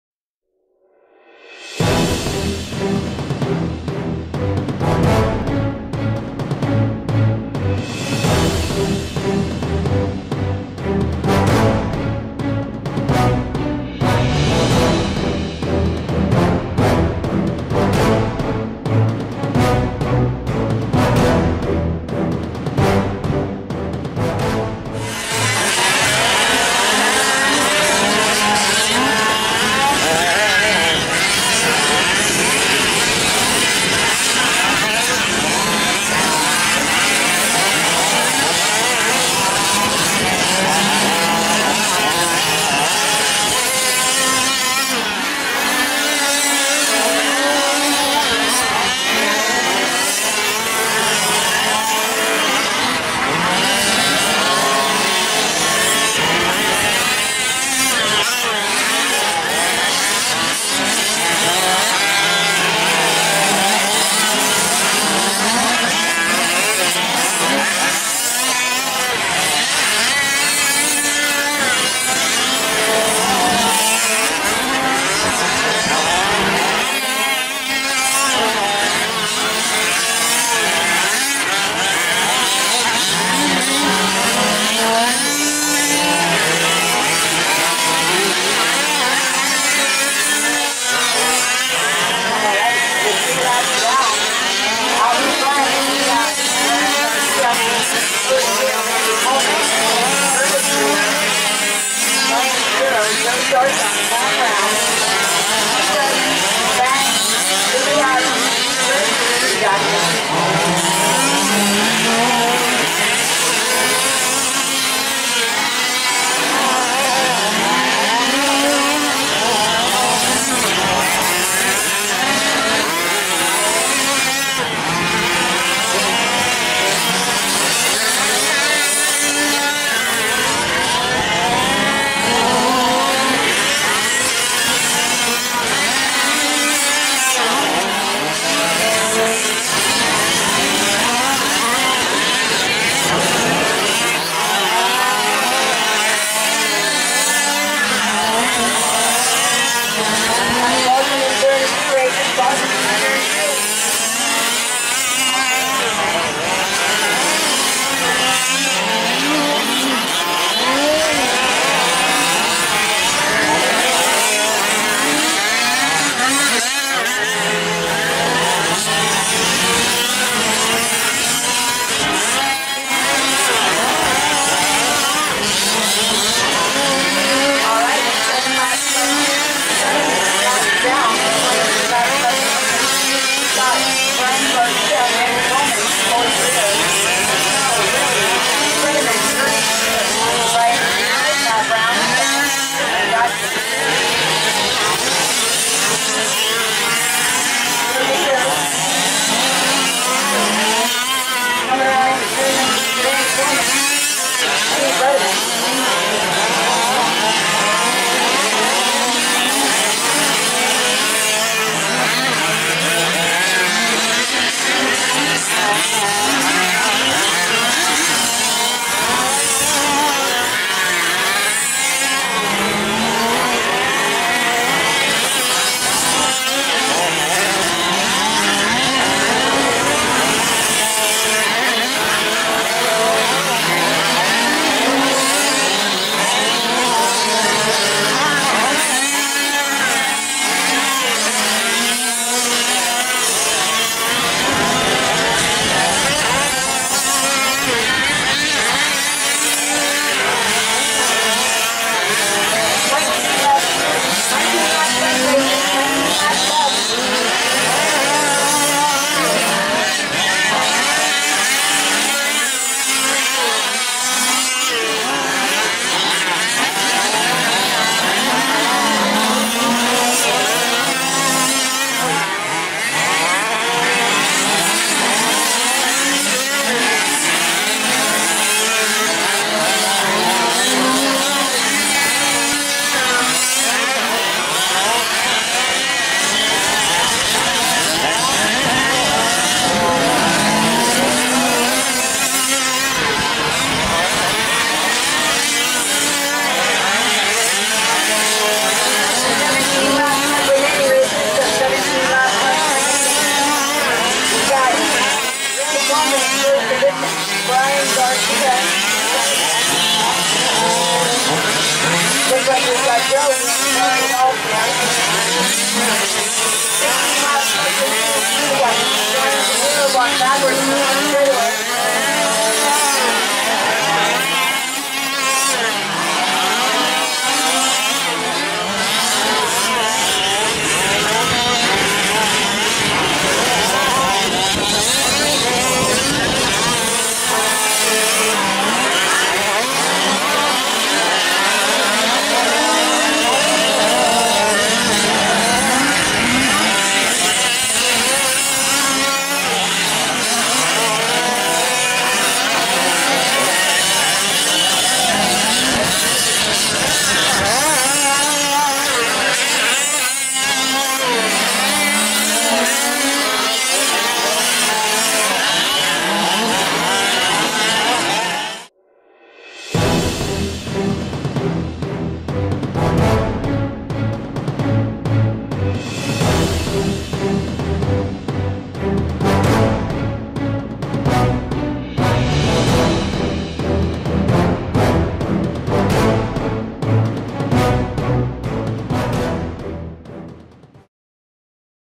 CORRS 2016 RD5: M8 4WD Truck Open - 7/23/16
Here is one of the videos from the racing action during Round #5 of the Chase Off-Road Racing Series (C.O.R.R.S.).

Thanks to Tasha Chase and Kevin Chase for once again being outstanding RC Hosts especially under the extremely hot conditions throughout the day. We survived the heat wave!!